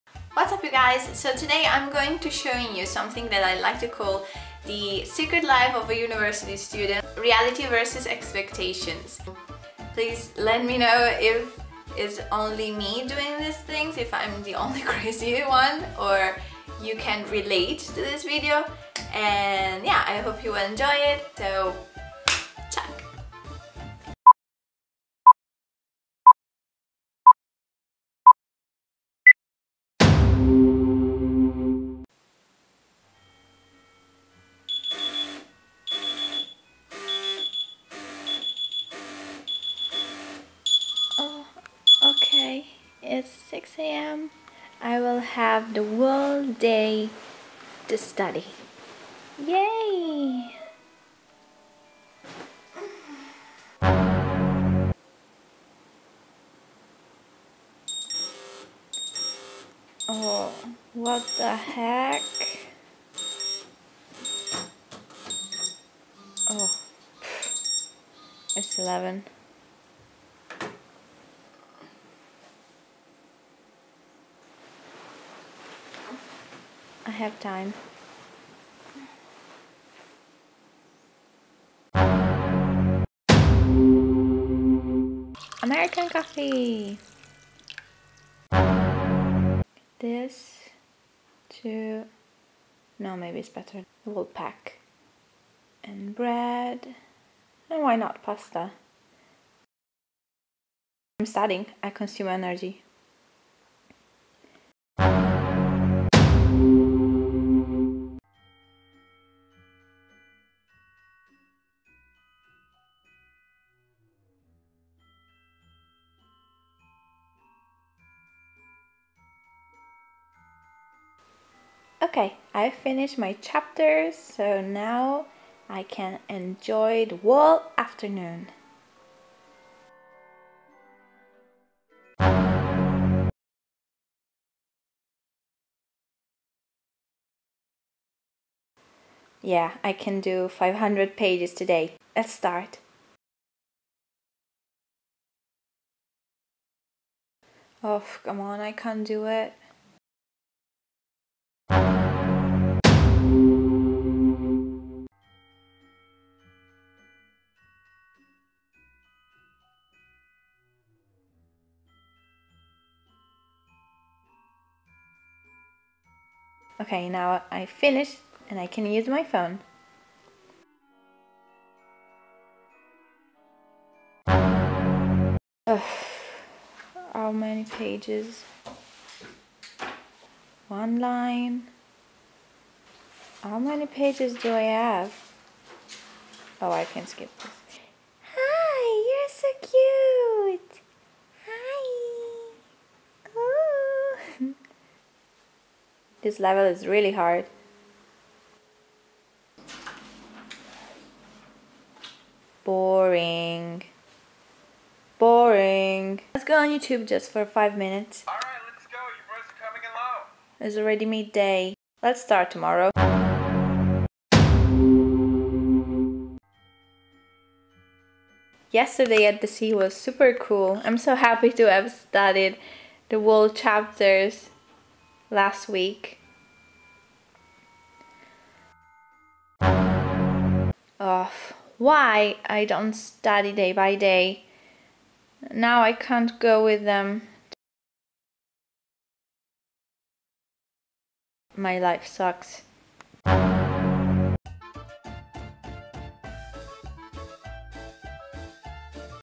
The Secret Life of a University Student: Reality Vs Expectations
Heyo, I wanted to make this video to see if other students like me do this things while studing, and if they procrastinate as well :)

Thanks

Jess x